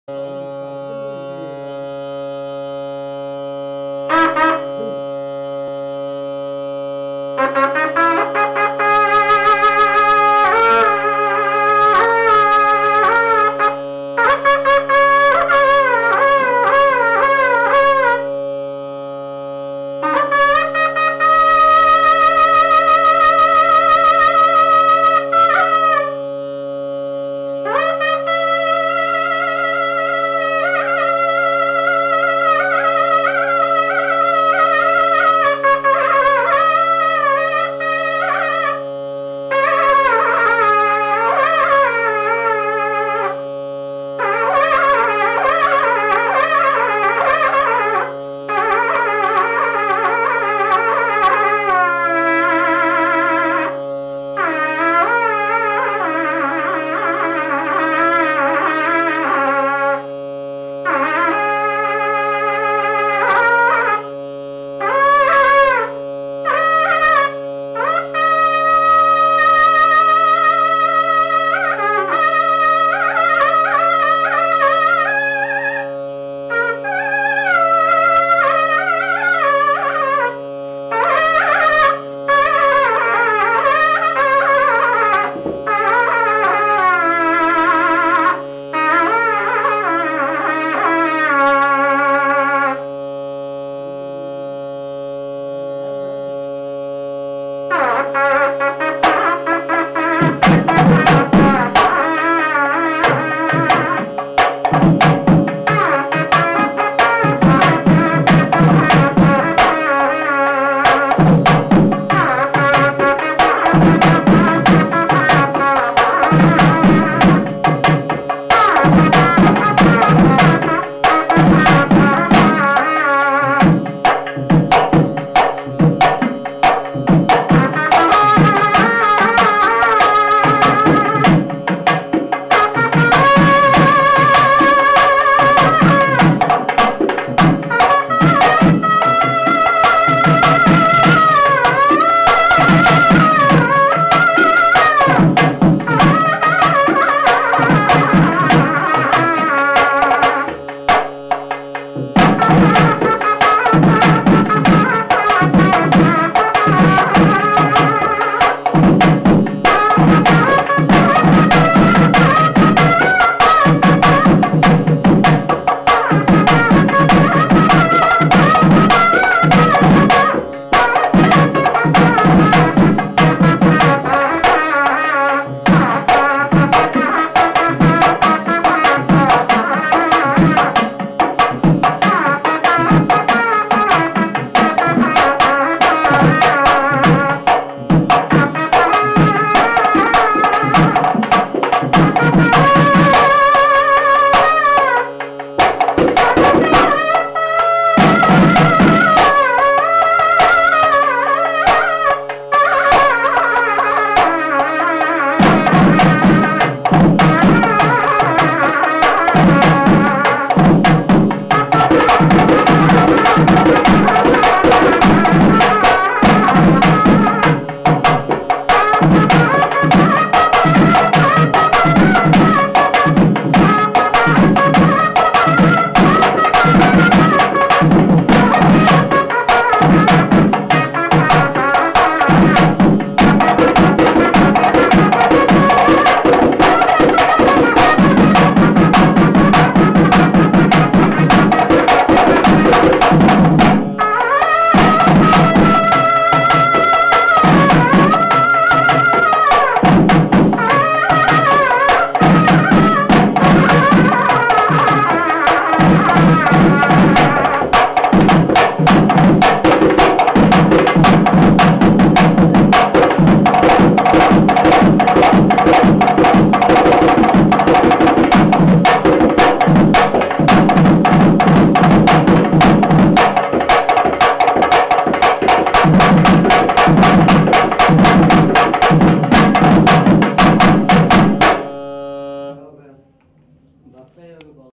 Gangi reddu Sannai - Part 5 - Annamayya Kalavedika
అన్నమయ్య కళావేదిక ‘‘గుంటూరు తిరుపతి’’ బృందావన్ గార్డెన్స్  శ్రీ వేంకటేశ్వరస్వామి వారి దేవాలయ ప్రాంగణంలోని  గంగిరెద్దుల మేళ సన్నాయి  ఆహుతులను ఎంతగానో  ఆకట్టుకుంది. మద్దిలేటి బృందం  ఆలపించిన పలు కీర్తనలను భక్తుల  ఎంతో శ్రావ్యంగా విని కళాకారులను ప్రోత్సహించారు.
దేవాలయ పాలకమండలి వారు కళాకారులను ఘనంగా సత్కరించారు.
నిర్వహణ: శ్రీ అన్నమయ్య  సేవాసమితి, గుంటూరు.